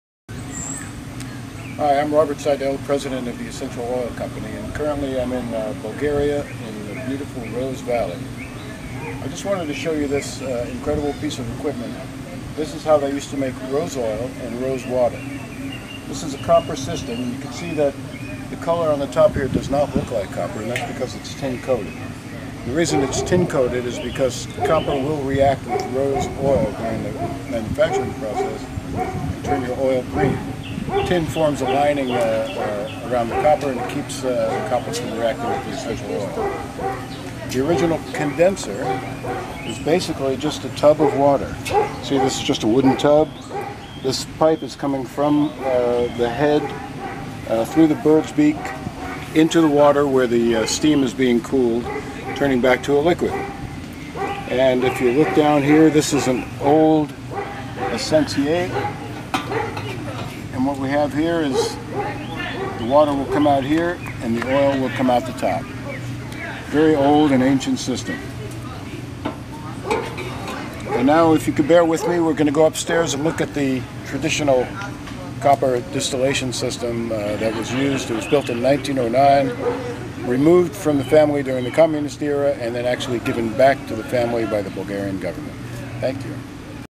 Ancient Distillation System For Rose Oil and Rose Water
Robert Seidel Founder of The Essential Oil Company explains the use of this Ancient distillation system can be found in Bulgaria.  It is a copper distillation unit with tin coating used to distill Rose oil and Rose water.  The roses are harvested from Rose Valley.